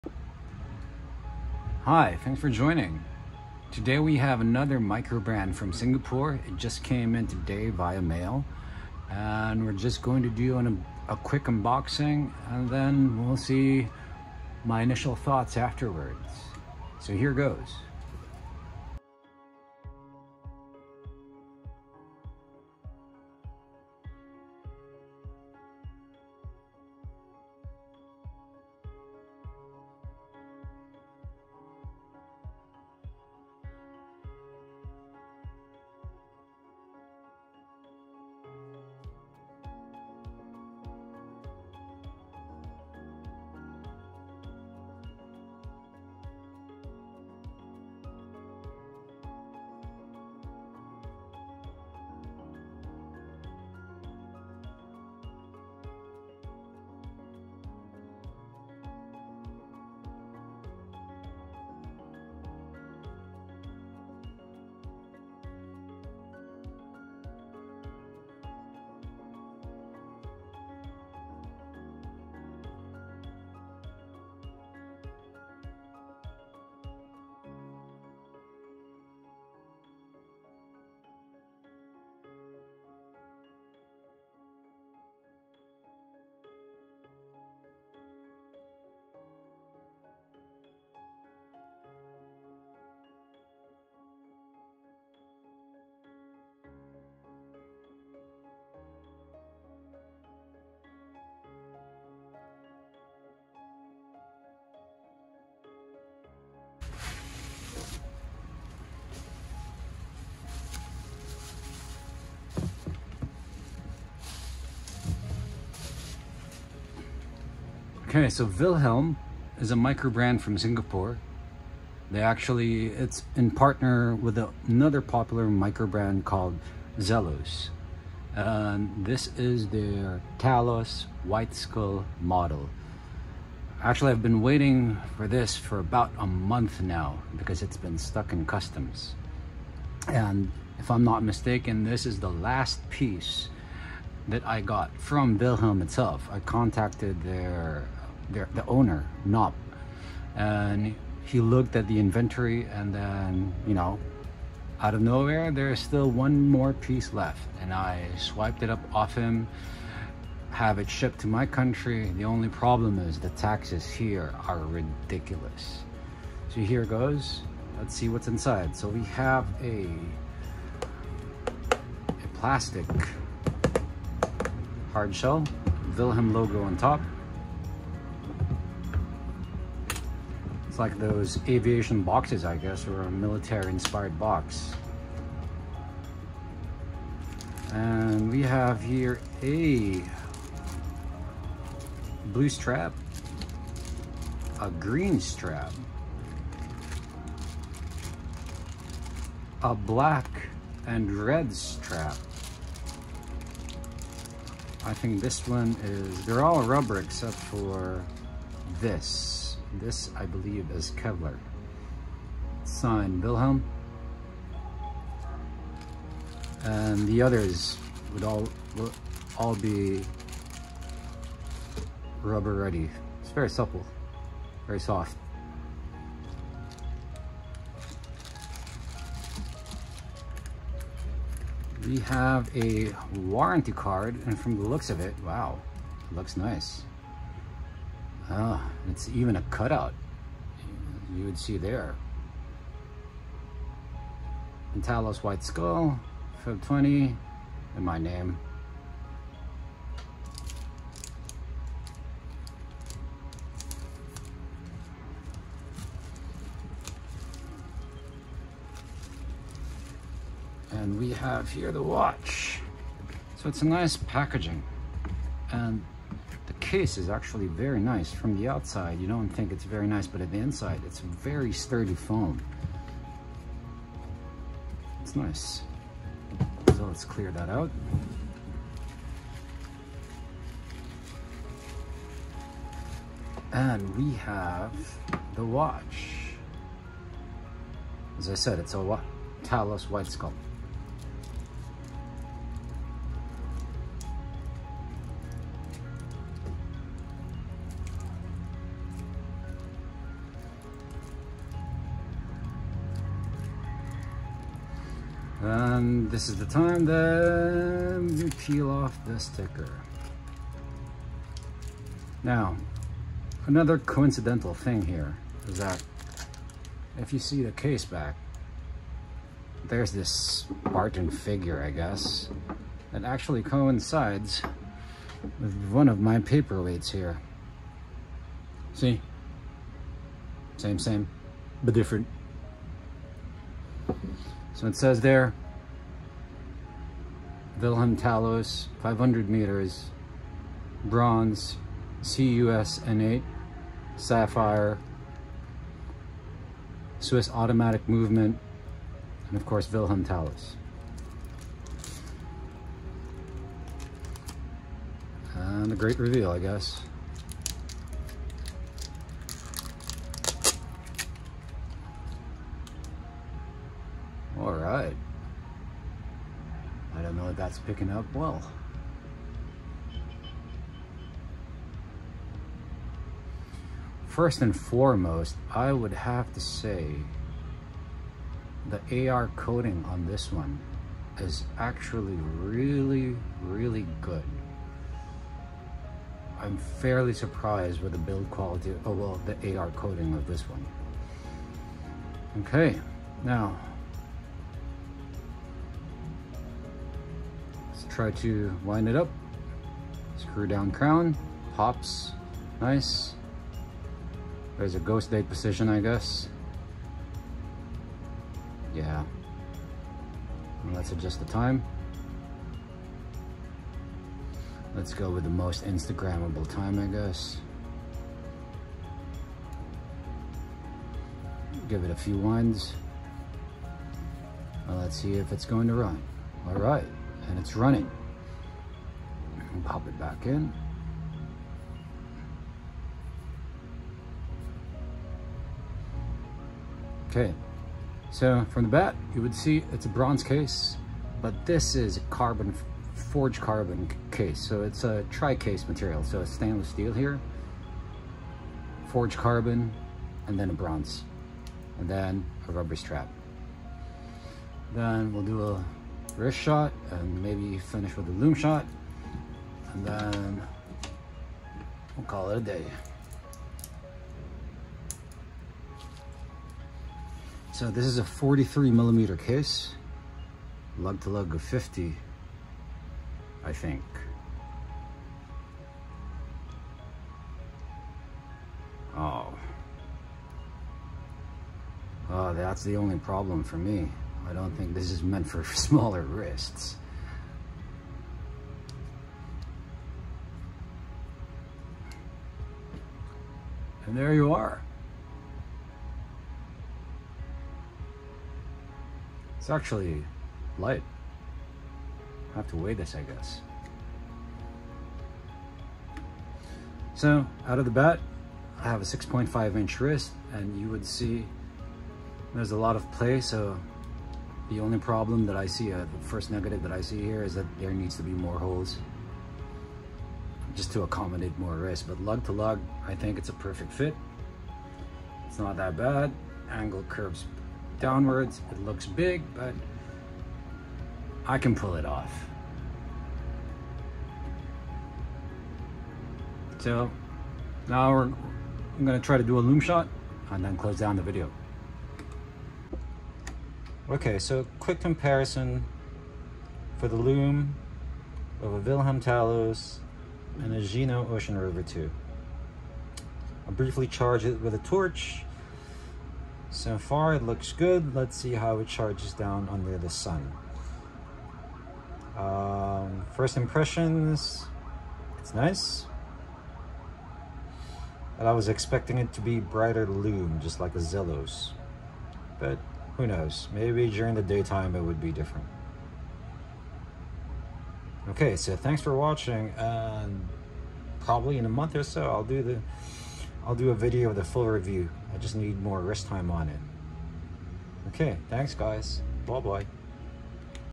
Unboxing the Vilhelm Talos White Skull Lume Dial
Unboxing a recent purchase and giving my initial thoughts